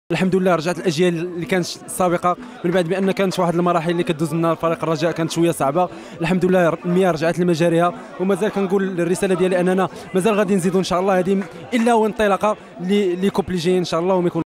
الرجاء_الرياضي_بطل_كأس_الكونفدرالية_الإفريقية2018|صلاح الباهي لاعب للرجاء الرياضي بعد التتويج بكينش
الرجاء_الرياضي_بطل_كأس_الكونفدرالية_الإفريقية2018|صلاح الباهي لاعب للرجاء الرياضي بعد التتويج بكينشاسا" عودة الثقة للفريق وما هذه إلا بداية لمشوار حافل" .